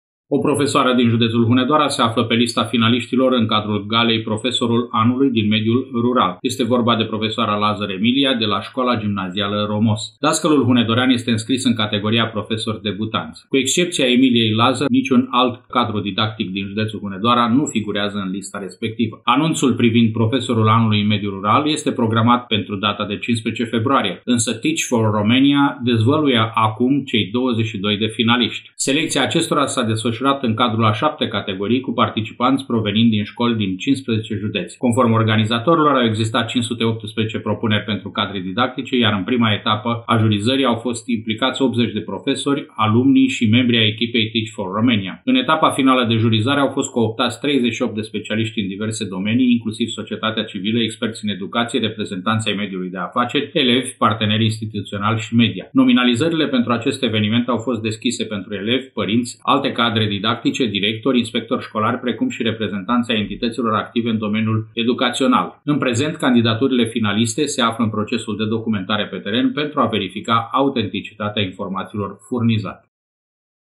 PROFESOARĂ DIN ROMOS PE LISTA FINALIȘTILOR
Emilia Lazăr poate deveni „Profesorul Anului în mediul rural”